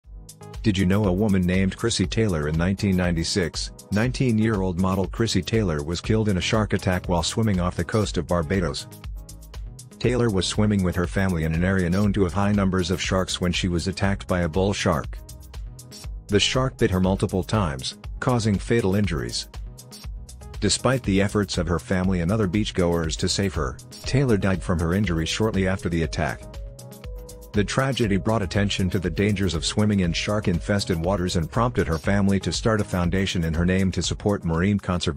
Shark Attack- Krissy Taylor
In 1996, 19-year-old model Krissy Taylor was killed in a shark attack while swimming off the coast of Barbados. This is a crazy story !